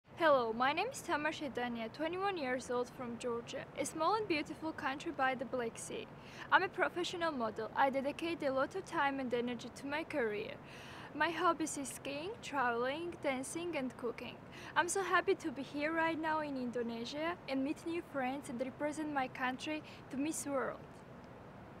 Miss World 2013 - Profile Video - Georgia
Meet Tamar Shedania, Miss Georgia World!